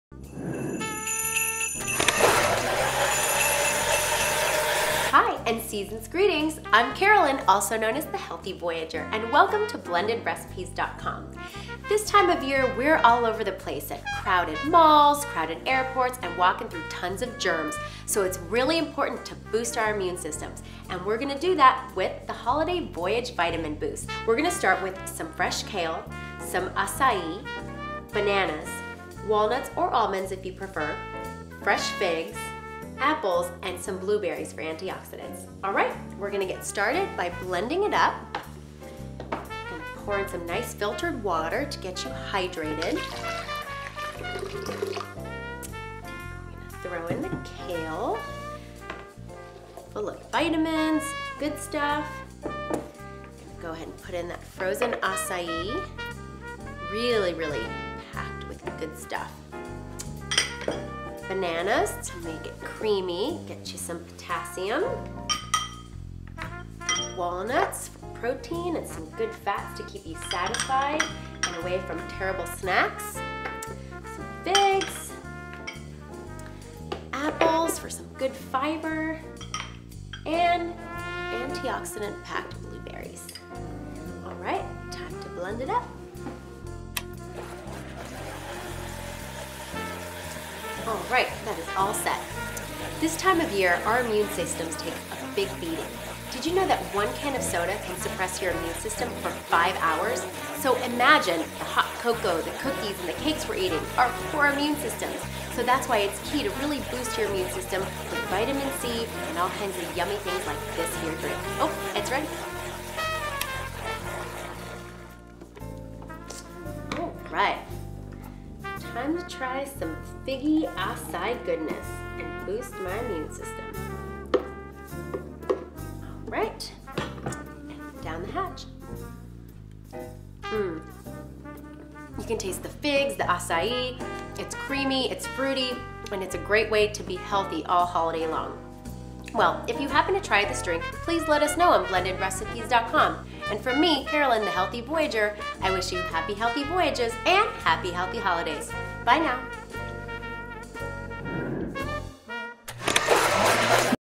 Holiday Voyage Vitamin Boost Smoothie
The Holiday Voyage Vitamin Boost Smoothie is a great way to get some of your most important vitamins in a delicious and quick way. The blend packs a healthy punch with kale, acai berries and figs, some of the most valuable fruits and vegetables in the world. The bananas and almonds help make the smoothie's creamy consistency and the apples and blueberries really bring the taste to life. Try one today!